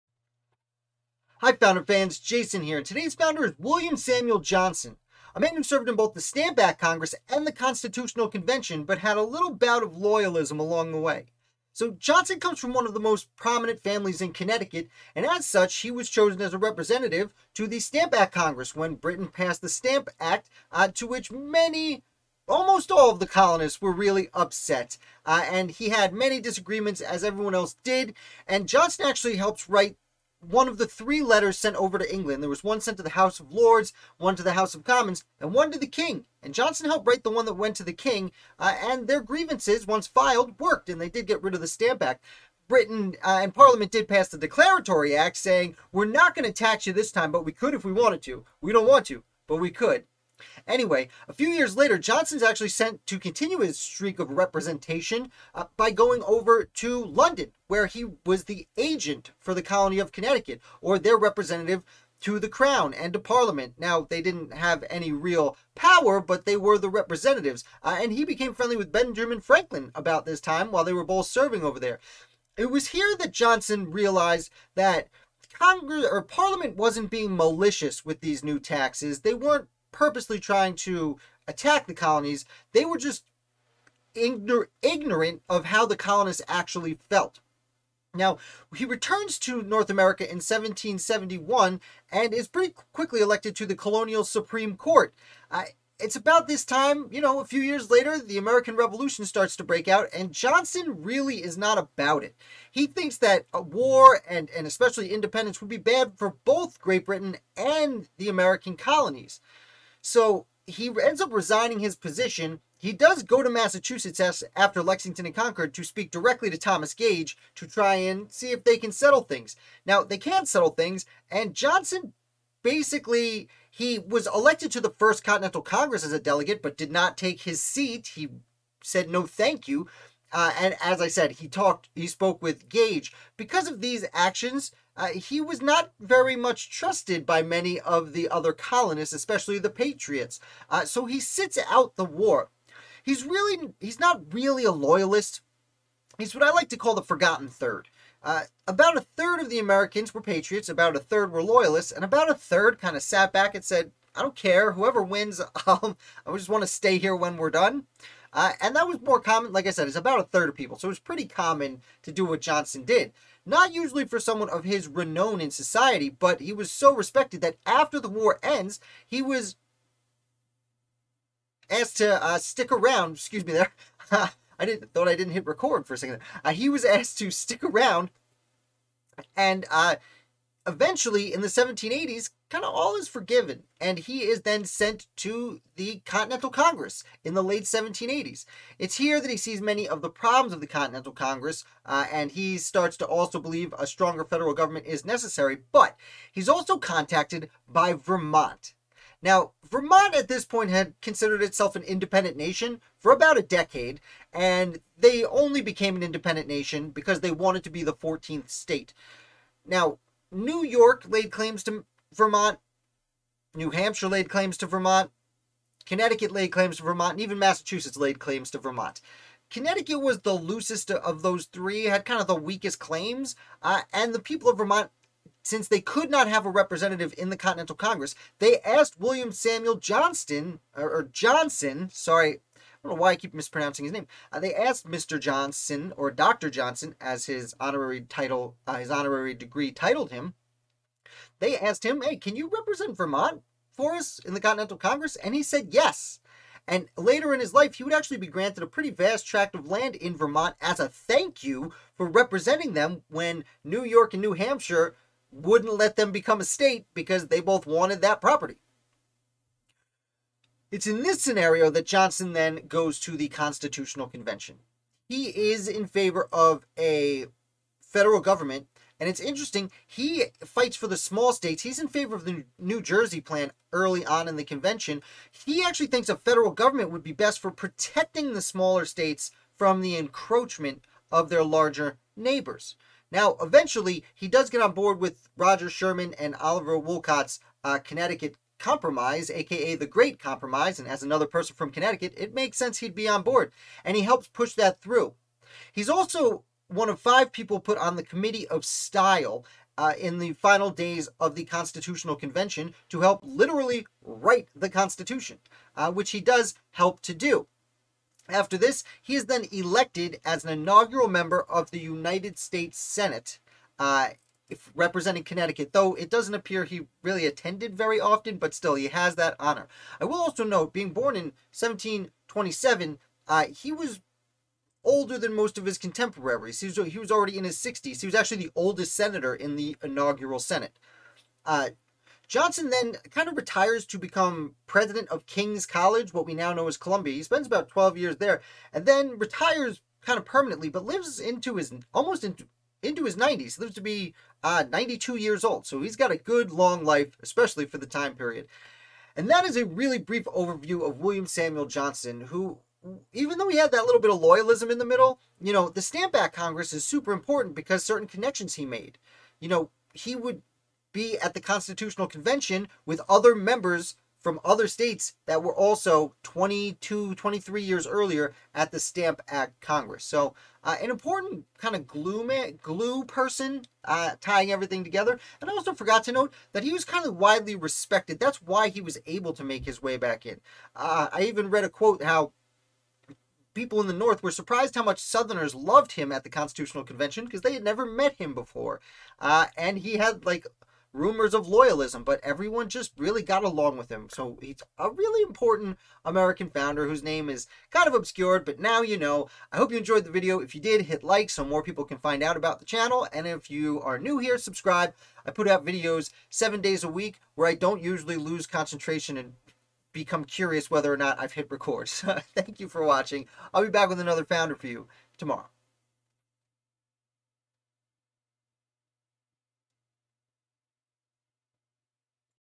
William Samuel Johnson - Temporary Loyalist
William Samuel Johnson was a Delegate to both the Stamp Act Congress and Constitutional Convention.